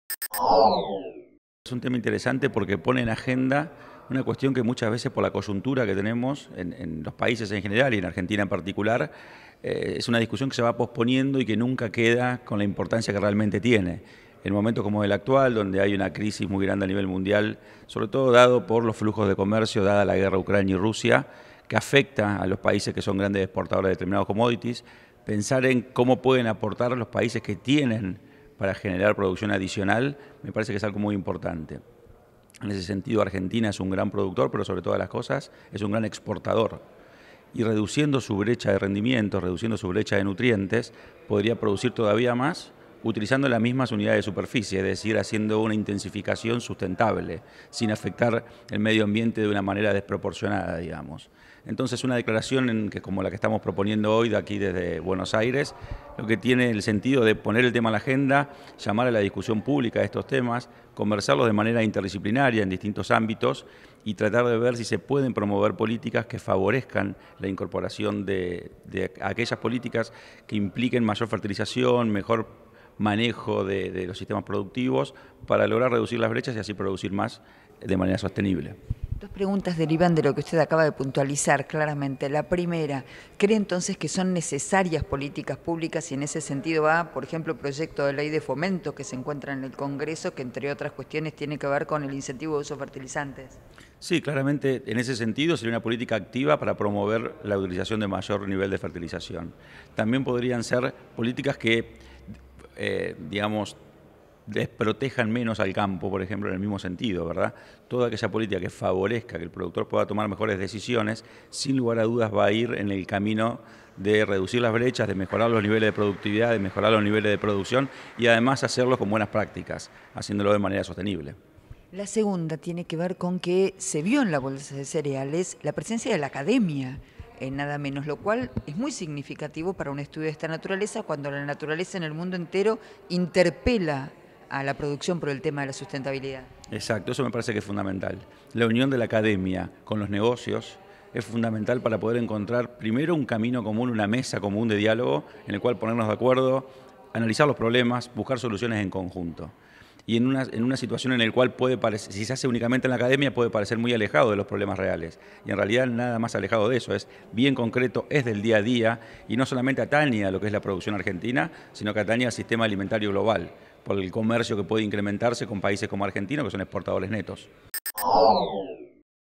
RAMIRO COSTA - ES MUY IMPORTANTE LA DECLARACIÓN SOBRE EL ROL DE LOS NUTRIENTES EN LOS SIST. DE PROD.
"ES MUY IMPORTANTE LA DECLARACIÓN SOBRE EL ROL DE LOS NUTRIENTES EN LOS SIST. DE PRODUCCIÓN DE CULTIVOS EN ARGENTINA"